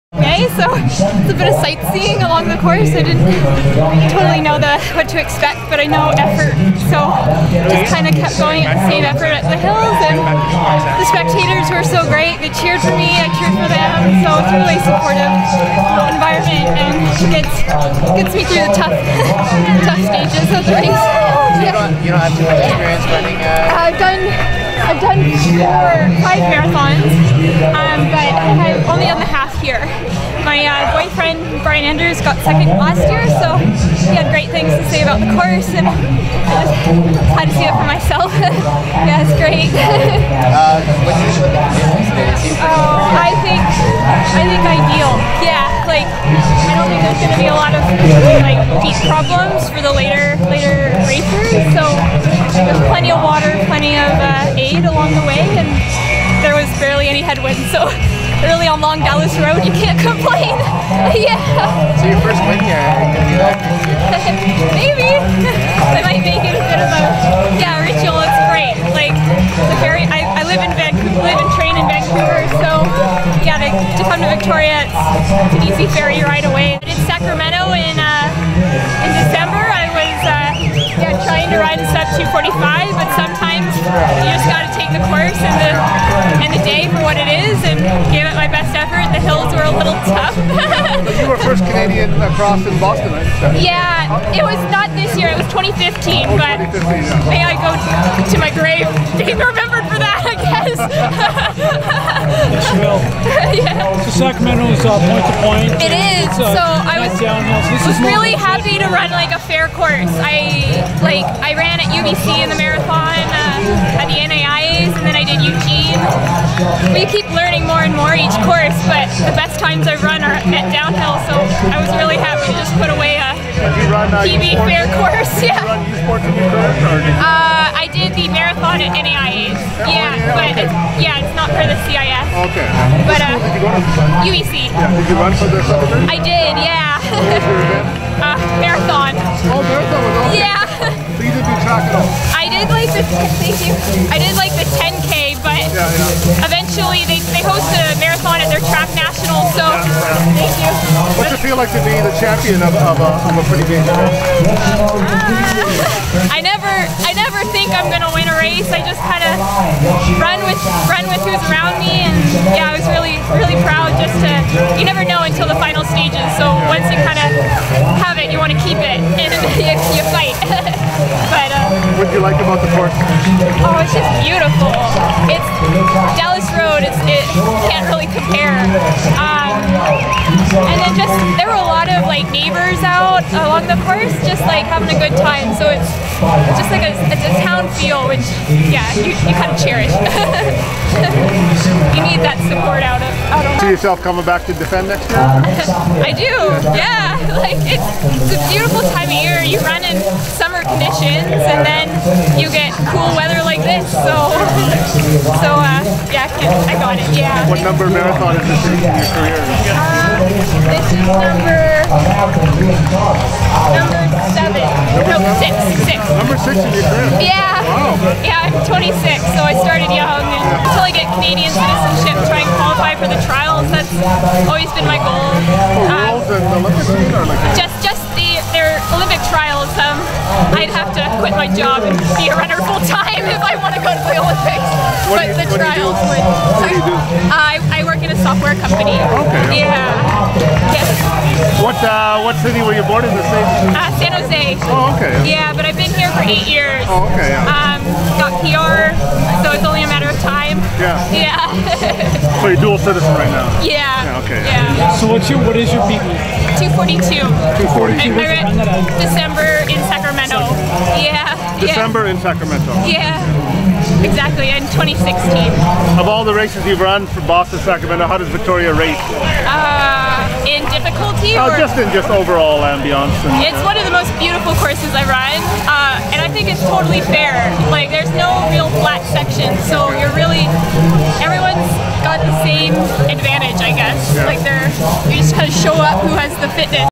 Lissa Zimmer interview - winner 2017 GoodLife Fitness Victoria Marathon
Lissa Zimmer interview - winner 2017 GoodLife Fitness Victoria Marathon, 2:48:33.